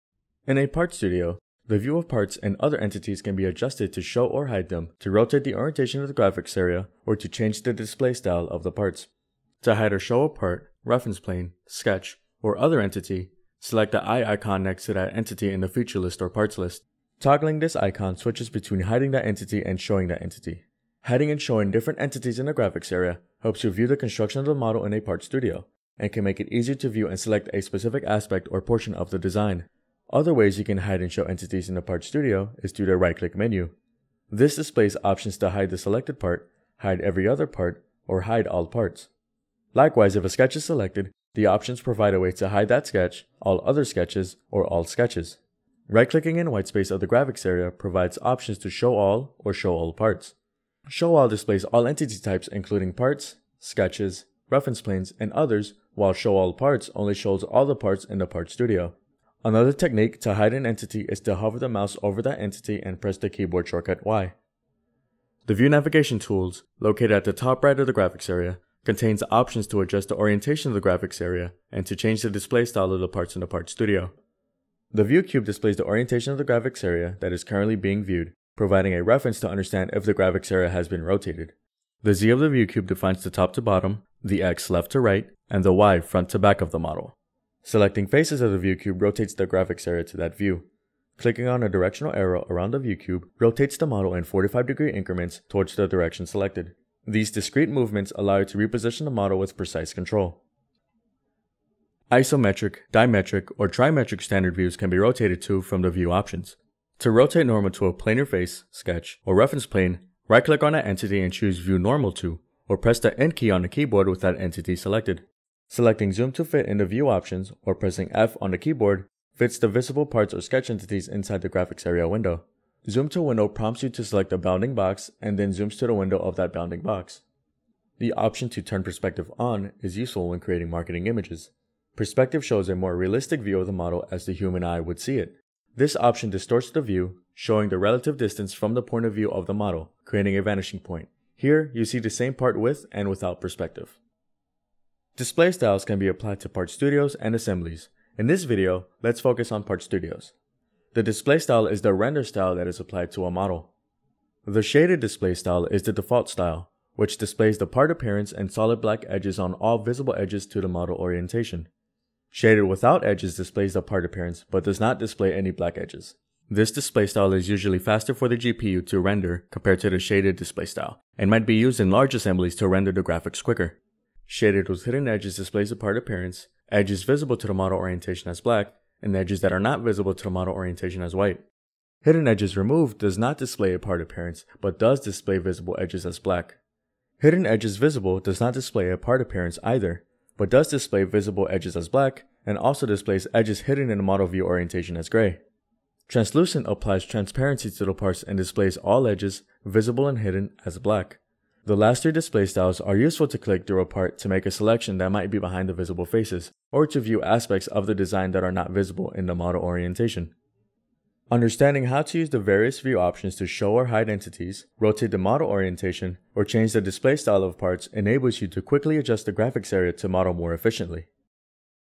Options to Show and Hide Different Entities
To fit the visible parts in a Part Studio to the graphics area window, use the keyboard shortcut F.
View options can be utilized to model more efficiently.